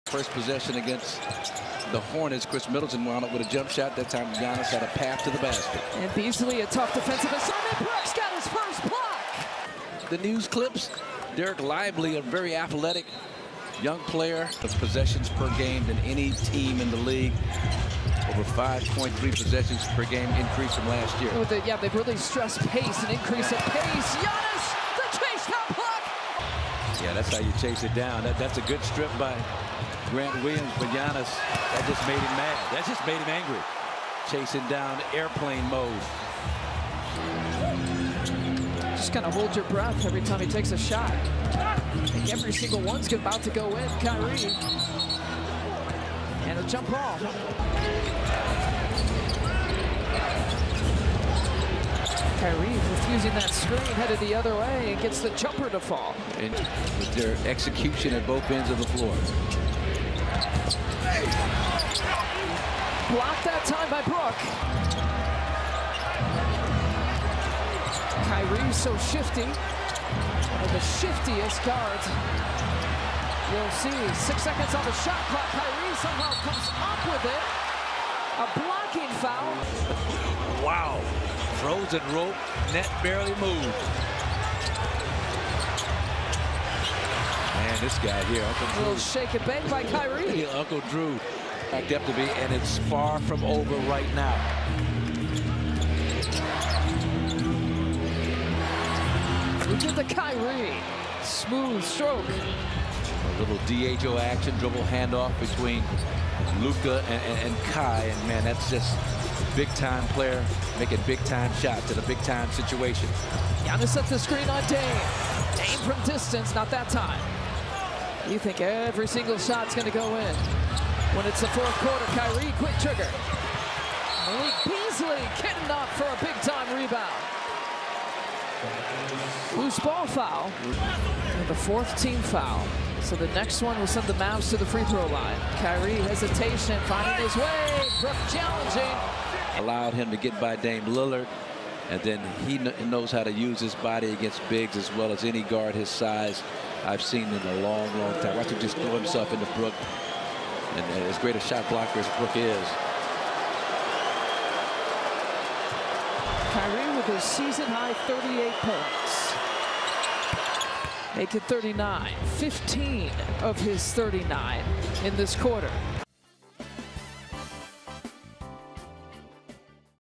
DAL Mavericks @ MIL Bucks 2023-24 NBA Regular season
Incredible competetiveness displayed by Kyrie Irving on both ends of the floor late in game

Defense and Hustle compilation by J.H.Moon November 18th 2023